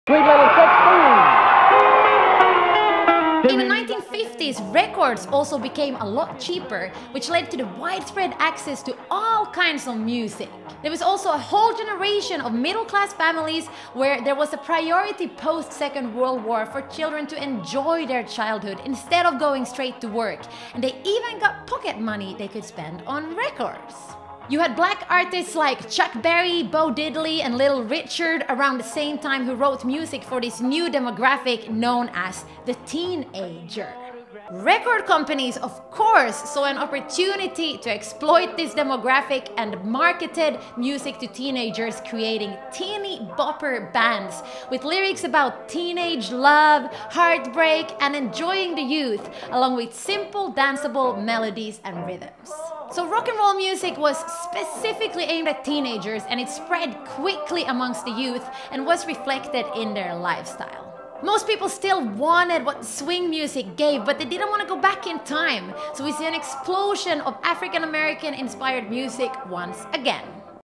Teeny-Boppers
This segment is taken from the 12 part series “Drumming Through The Decades”.

Drumming Through The Decades is created as an educational platform that covers the evolution of the drum kit, rhythms and genres, whilst highlighting female identifying drummers and musicians.
The series starts 2000BC, when all drummers were women, and continues all the way up to 2021. The purpose of the series is to present the female perspective that has often been left out in the history of music, and illuminate the stories of incredible drummers, many who have been forgotten today.